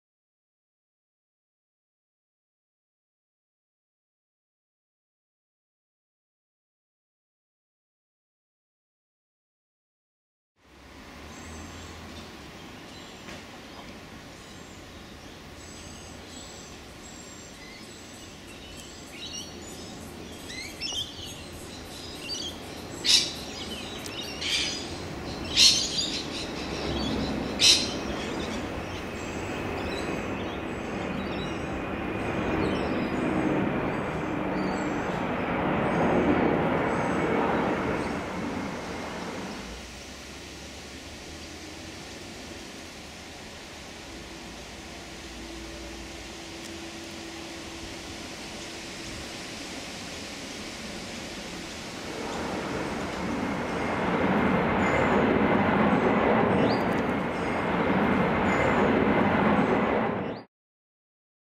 Orchard Swallowtail Butterfly on citrus tree - Papilio aegeus
I was heading off to the shops when I noticed this butterfly...Put my shopping list away and whipped out the camera... :)

Orchard Swallowtail Butterfly on citrus tree
Papilio aegeus
Enmore NSW
Friday 23 September 2022
11:11 am